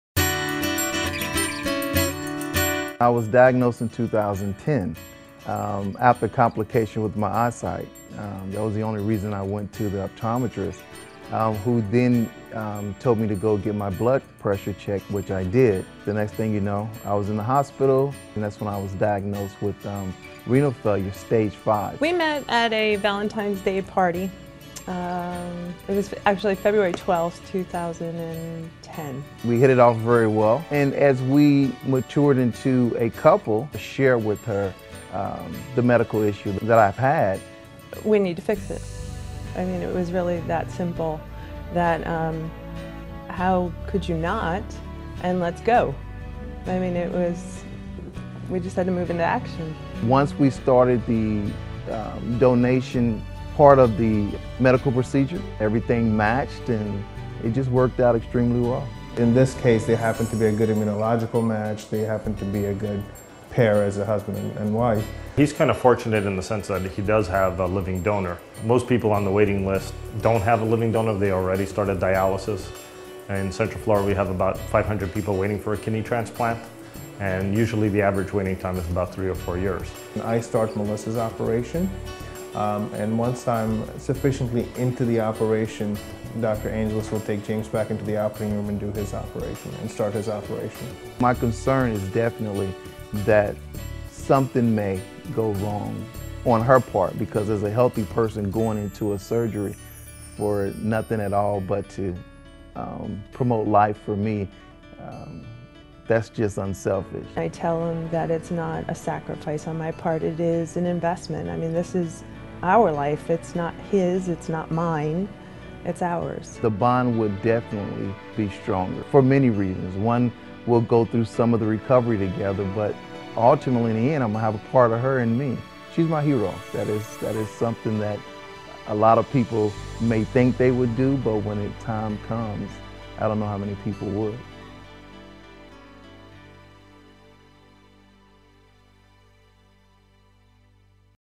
Living Donor Valentine's Gift - A Kidney and Life
Most couples in love celebrate Valentine's Day with a nice dinner out or maybe just a box of chocolates or flowers, but Melissa DeGeso-Jones is turning the traditional Valentine's gift on its head.  She donated a kidney to her husband just days before Valentine 's Day.  The two newlyweds from Lake Mary, FL will spend the holiday of love recovering from surgery.